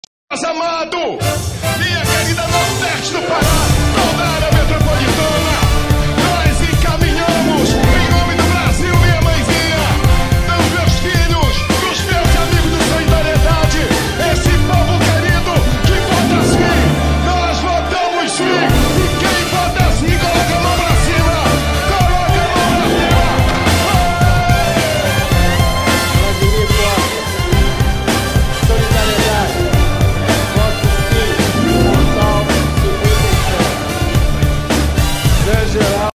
This Republicans Of Brazil Is Hero
Deputados Federais Foram Votados
Aos 5 Furiosos E o Dragão Guerreiro

Clipe Com Trilha Sonora De Hans Zimmer & John Powell